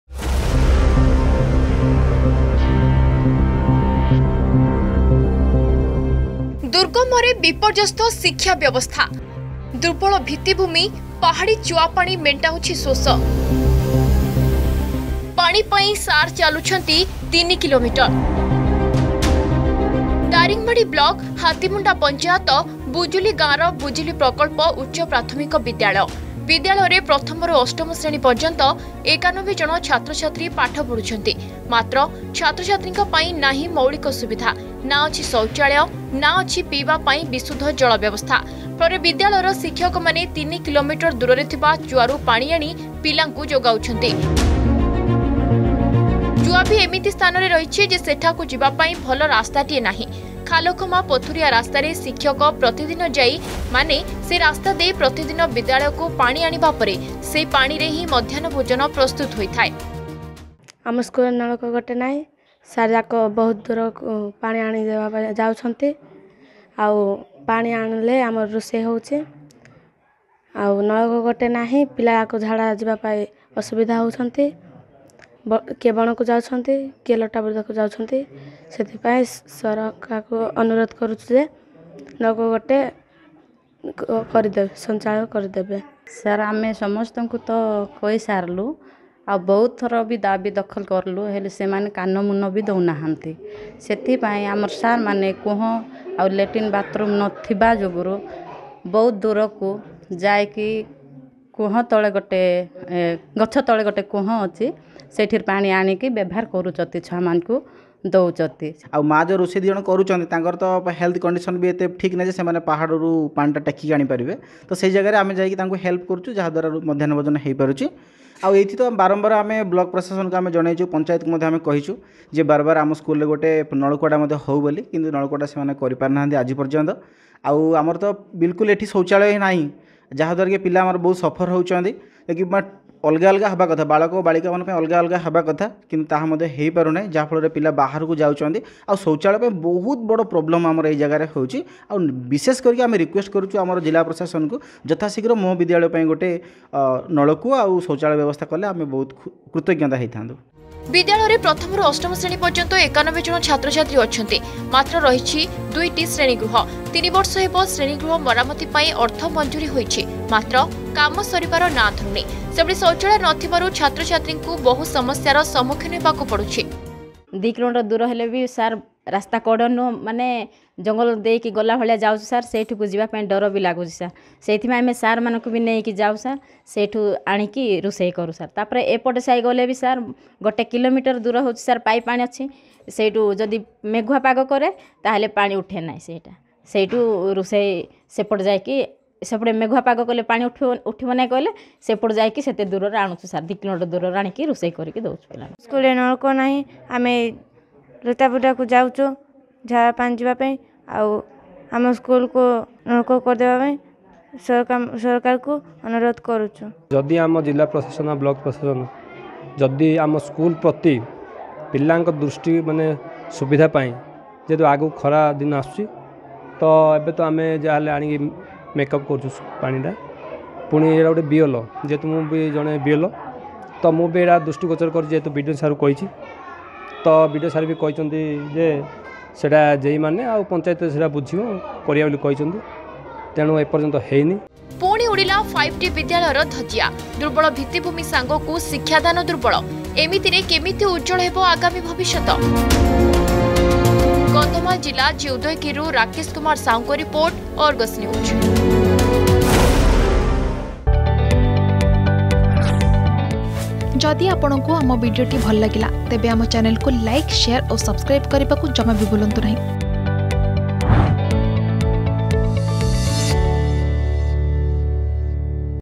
ଦୁର୍ଗମରେ ବିପର୍ଯ୍ୟସ୍ତ ଶିକ୍ଷା ବ୍ୟବସ୍ଥା || Daringibadi school problem
ଦୁର୍ବଳ ଭିତ୍ତିଭୂମି, ପାହାଡି ଚୁଆ ପାଣି ମେଣ୍ଟାଉଛି ଶୋଷ । ପାଣି ପାଇଁ ସାର୍ ଚାଲୁଛନ୍ତି ୩ କିଲୋମିଟର । ଦାରିଙ୍ଗବାଡି ବ୍ଲକ ହାତୀମୁଣ୍ଡା ପଞ୍ଚାୟତ ବୁଜୁଲି ଗାଁର ବୁଜୁଲି ପ୍ରକଳ୍ପ ଉଚ୍ଚ ପ୍ରାଥମିକ ବିଦ୍ୟାଳୟ  l